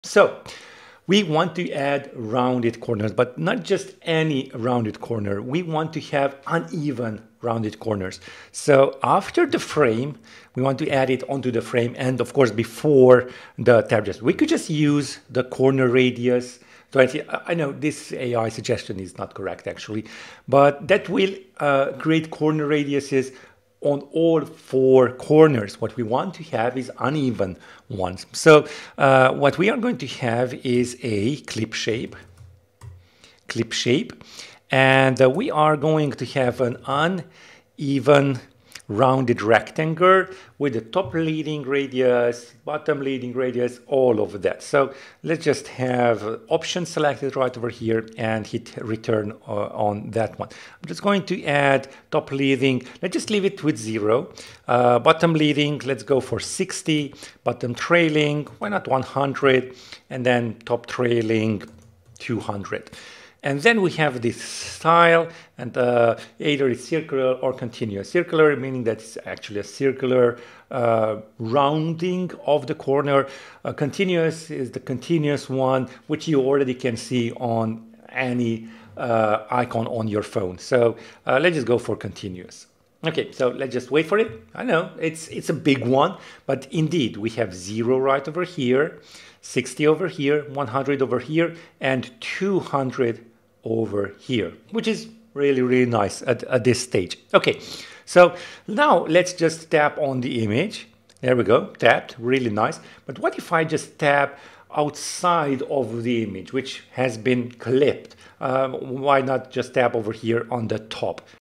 This SwiftUI Rounded Corner Trick Fixes Broken Tap Areas
Your SwiftUI UI can look perfect, until tap gestures quietly stop working.
In this video, I break down a subtle rounded-corner issue that causes wrong tap areas, broken gestures, and confusing UX and how to fix it properly.
Christmas Sale is live 🎄